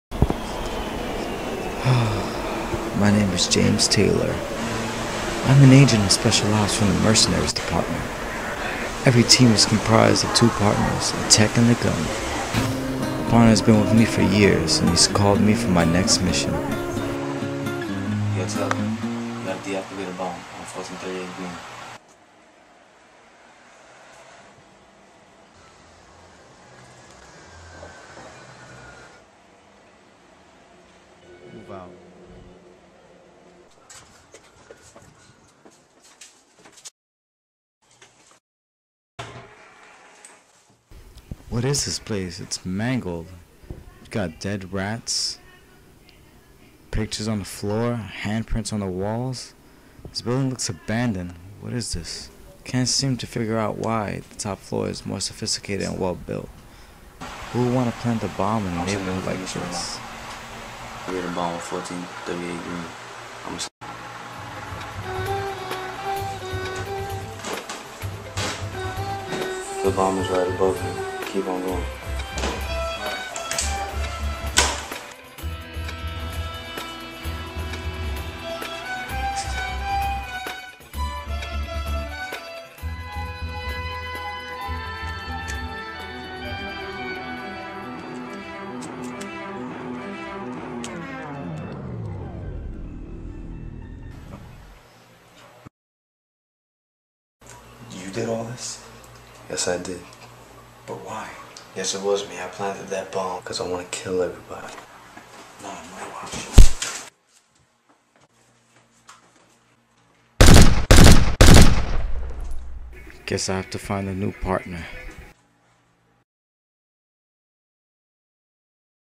Another Dull Day for James Taylor
A small clip of an agent from the mercenaries department. James Taylor the "gun" and his partner Mickey Rodriguez the "tech". A bomb needs to be defused before it destroys people in a 2mile radius. By Artie Sanchez from Reelization and ERDA.